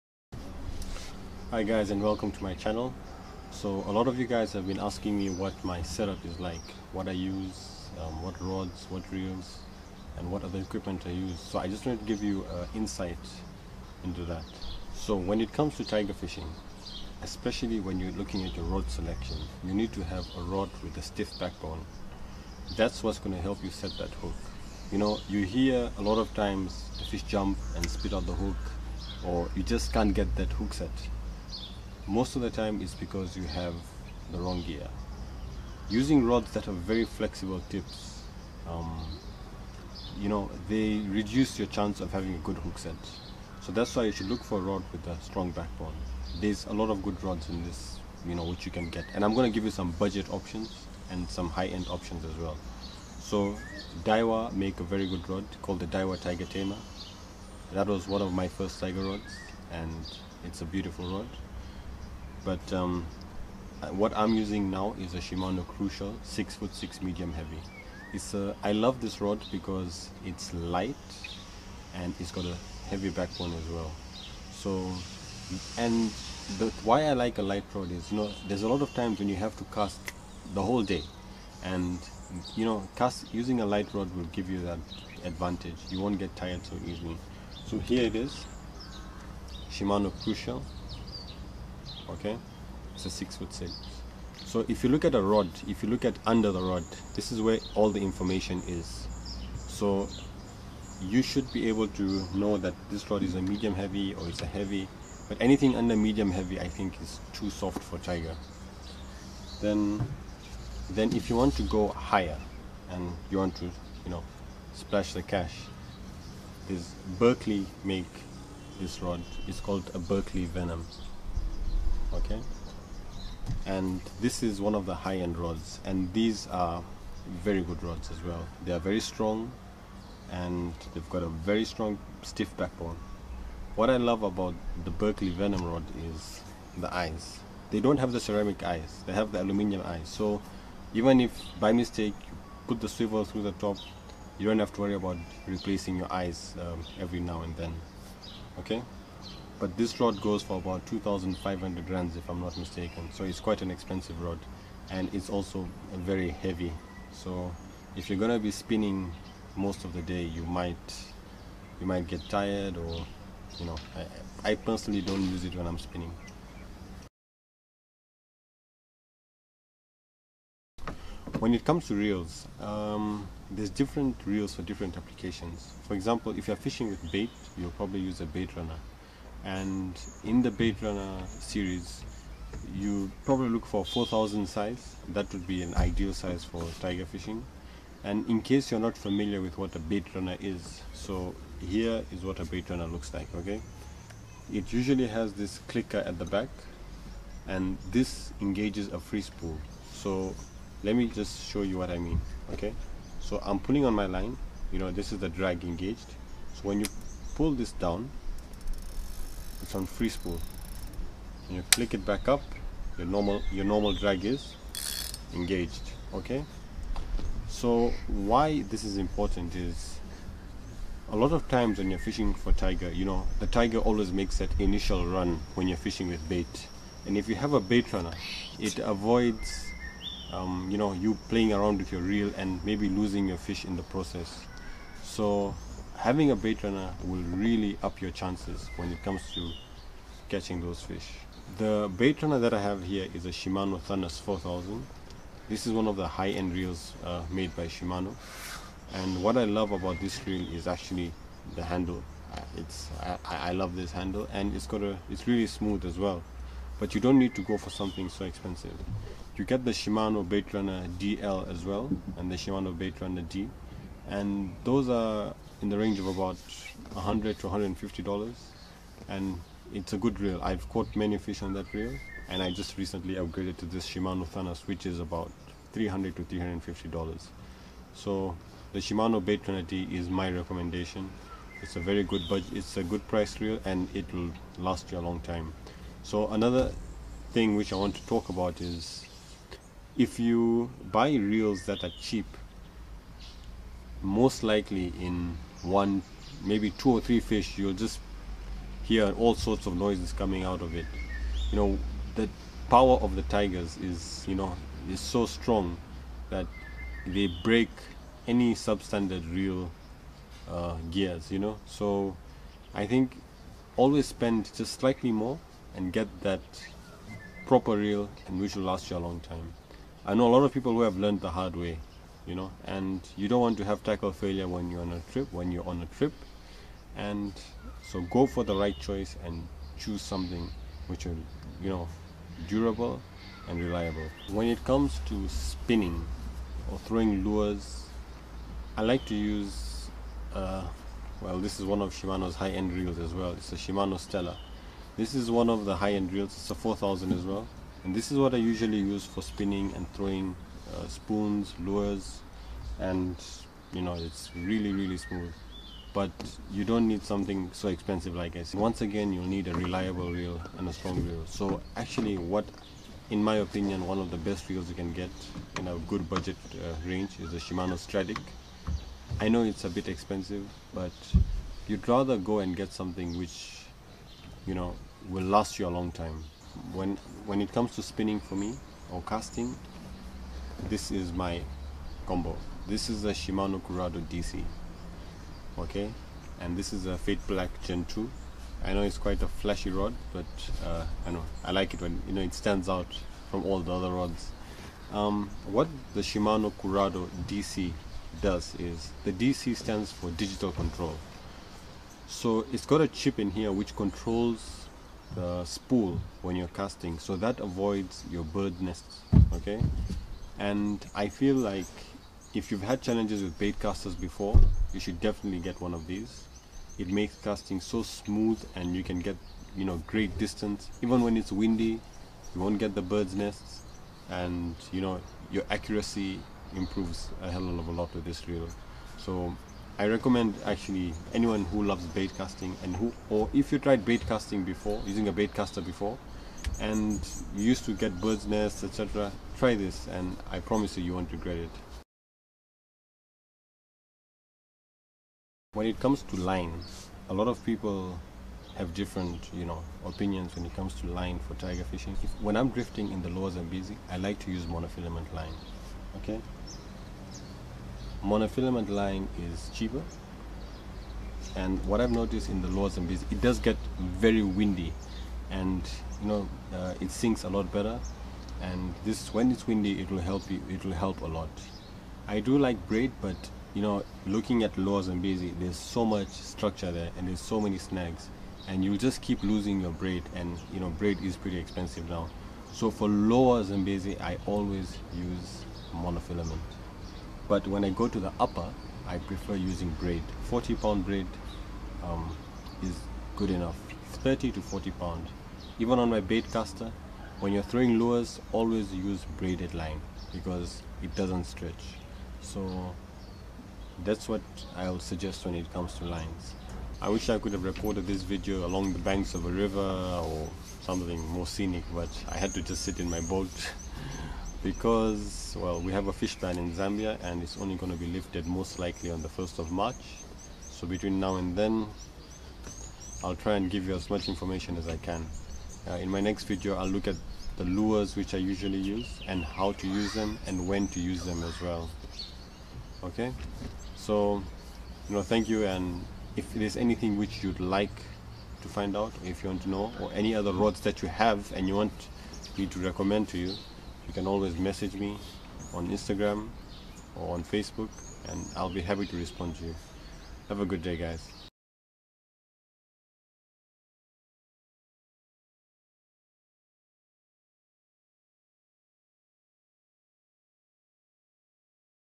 My Tigerfish Rod and Reel Selection
Here’s an insight on what tackle I use,
Most of my equipment is in the mid to high range because of the amount of fishing I do; I realized it’s better to spend abit more and have something that will last me many years rather than constantly changing gear. However this year I will review a lot of low budget options.
Thanks!